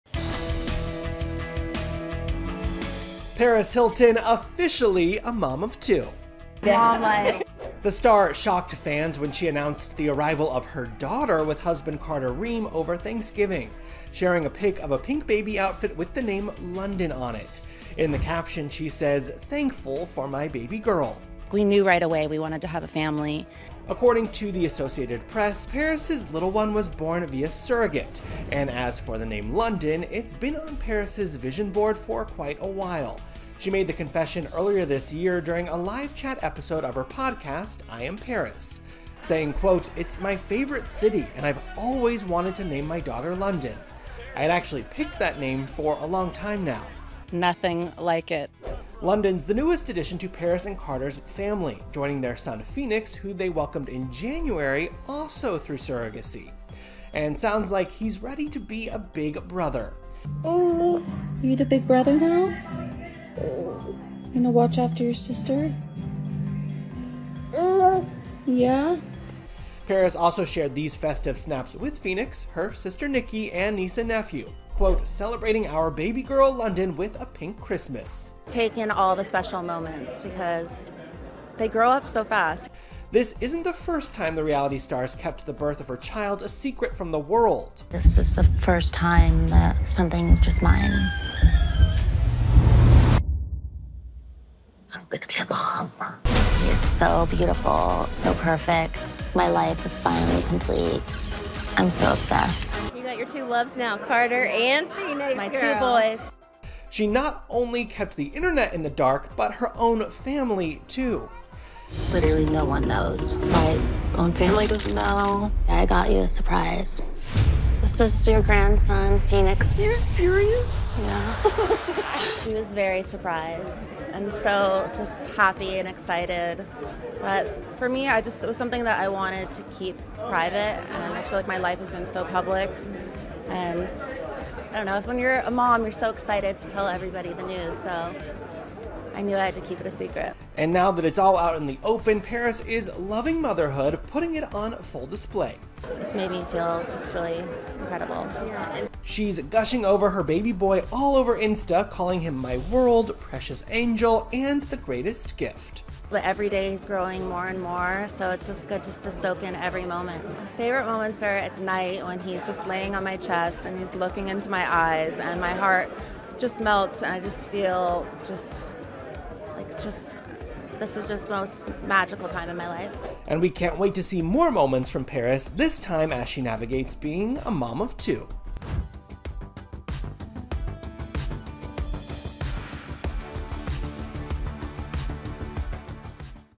Paris Hilton SECRETLY Welcomes Baby No. 2 With Husband Carter Reum
Paris Hilton becomes a mom of two after secretly welcoming her second child, a daughter, with husband Carter Reum. She announced the news on Thanksgiving, posting a photo of a pink baby outfit with the name, 'London,' embroidered on it. The couple's baby girl was born via surrogacy, according to Associated Press, as was their first child, son Phoenix, in January.